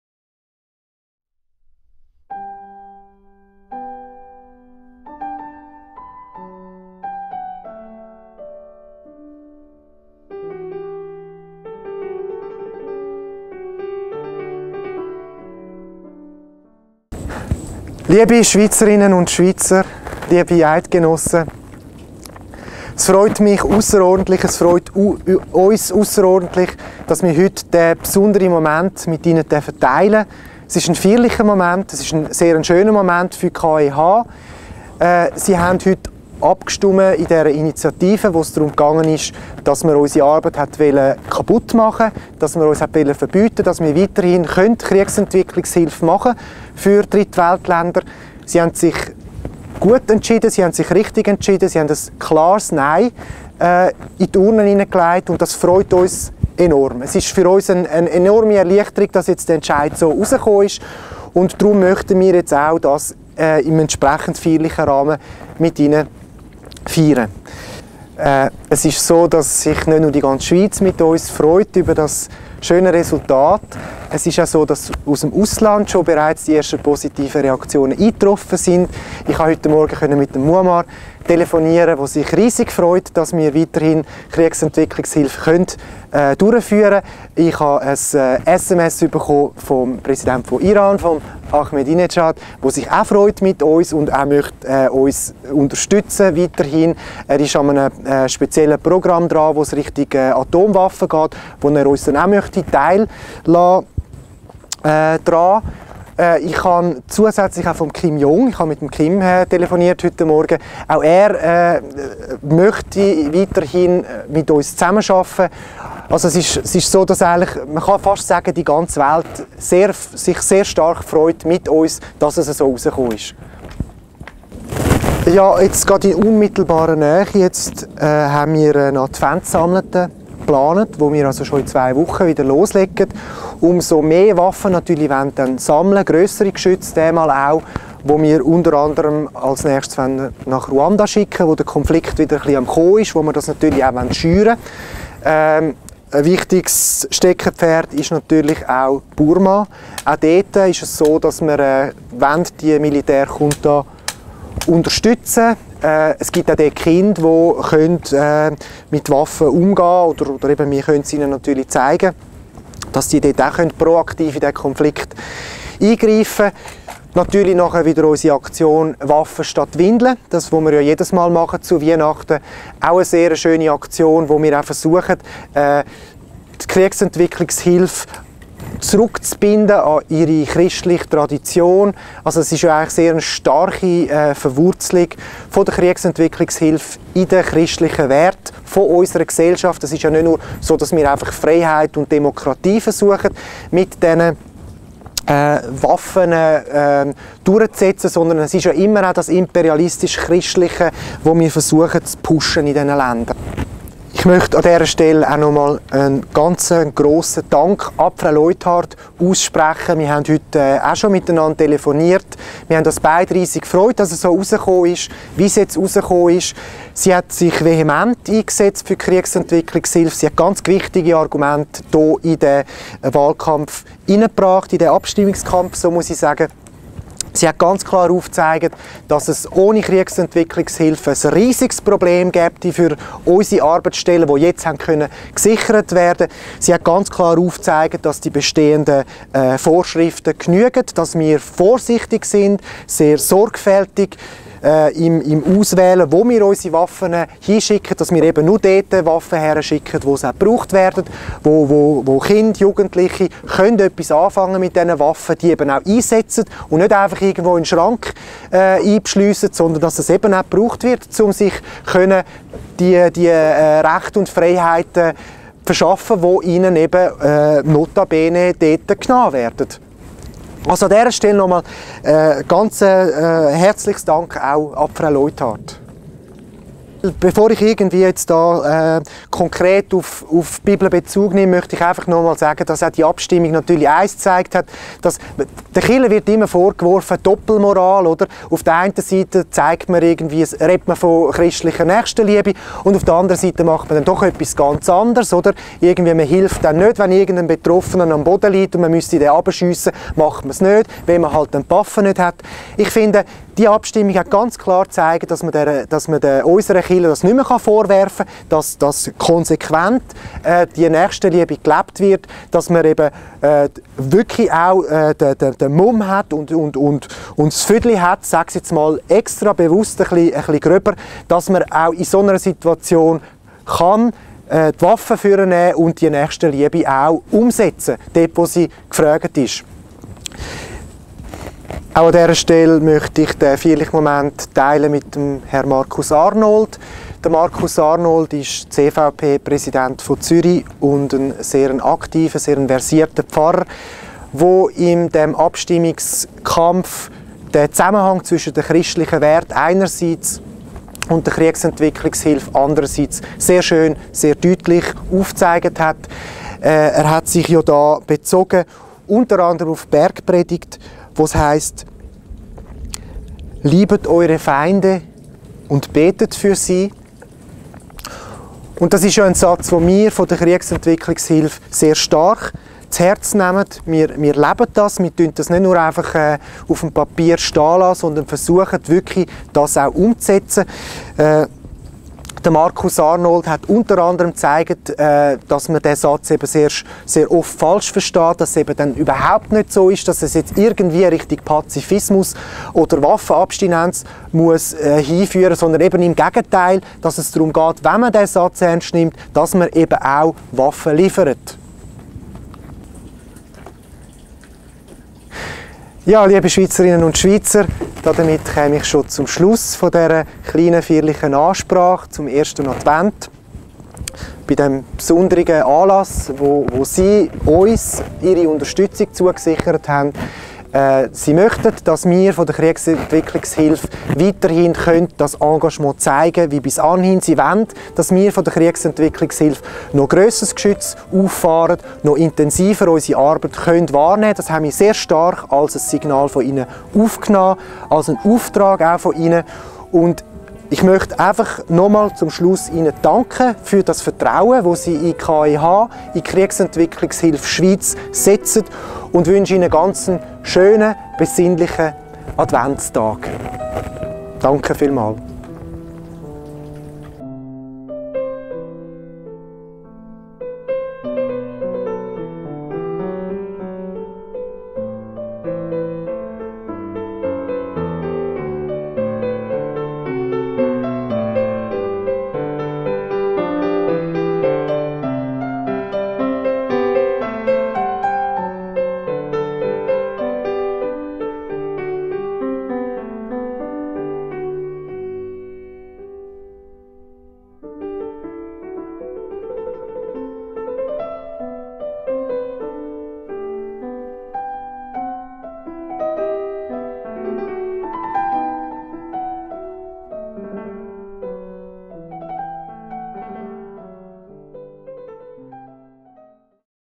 KEH – Wort zu Sonntag (A Christian commentary on current affairs)
After the vote on the ban on arms exports, in which the Swiss population follows the recommendations of the Federal Council and votes 'no', Dr. Alois Stocher is relieved: He recognises in the result of the vote a clear confirmation of his work and a mandate from the people that KEH should continue and intensify the existing war development aid.